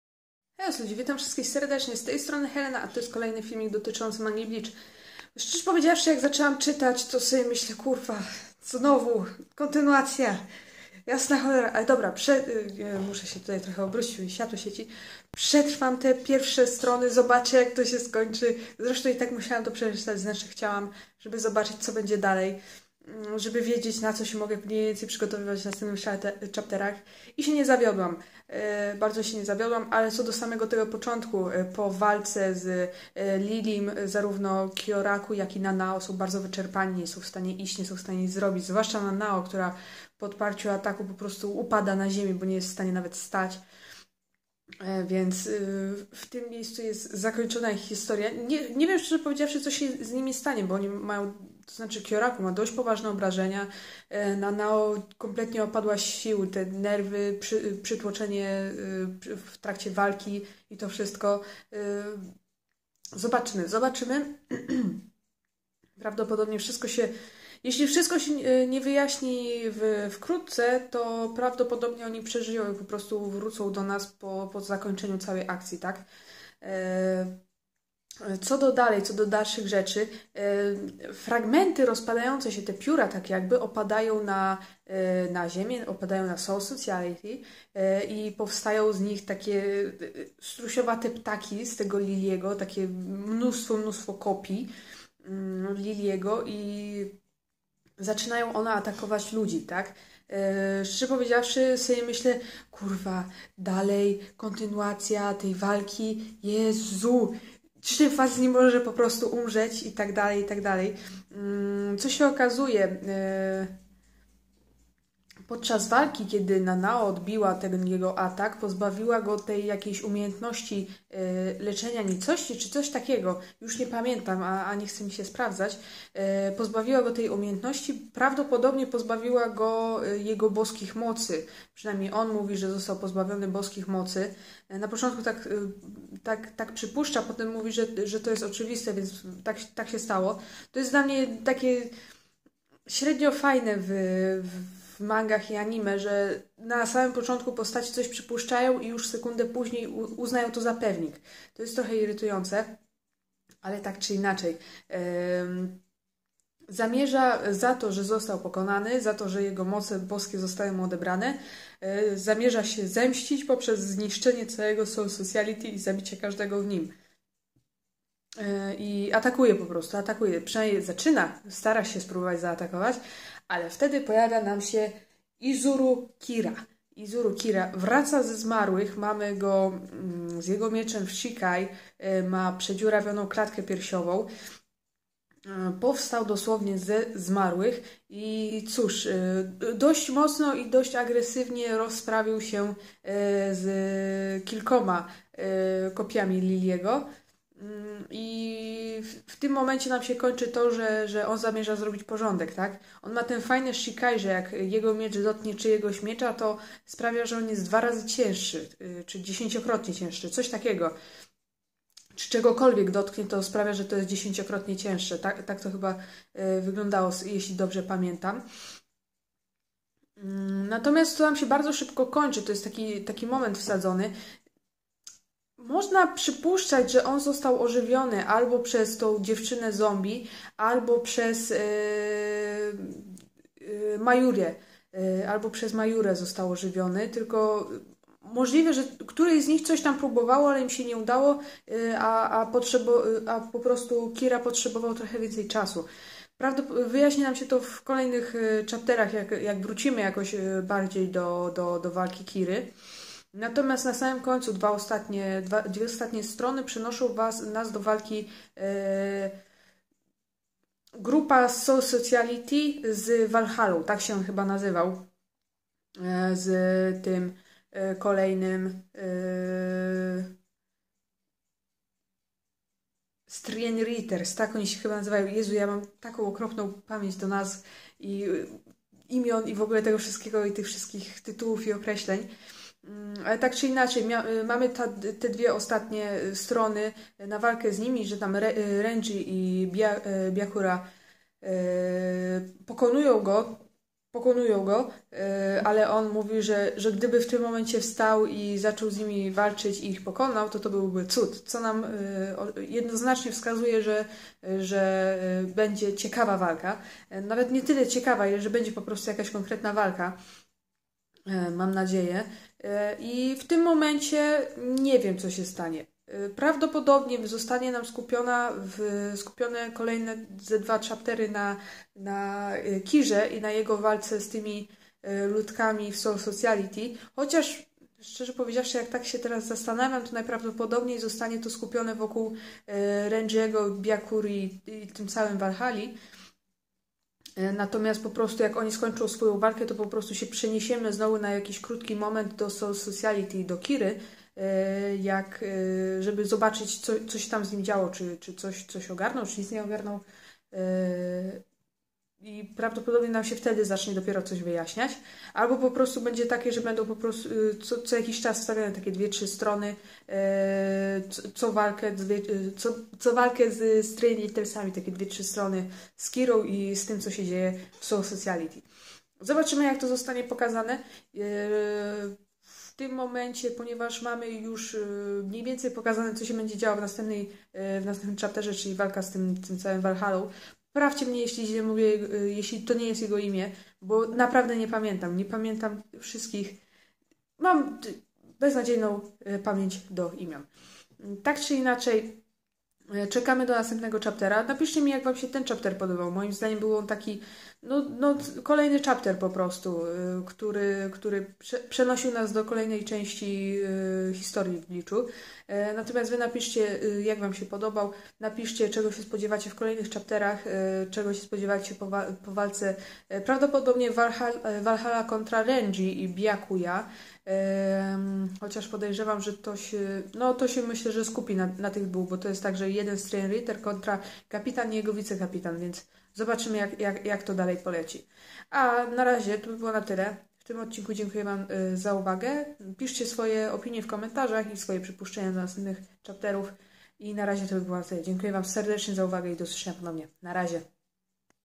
Bleach Manga Chapter 654 Ocena Lille po prostu nie może odpuścić .-.
FILM ZAWIERA SPOILERY!
Po wyczerpującej i długiej potyczce między Kyoraku i Nanao, z Lillem, w końcu ruszamy dalej, jednak nie tak do końca.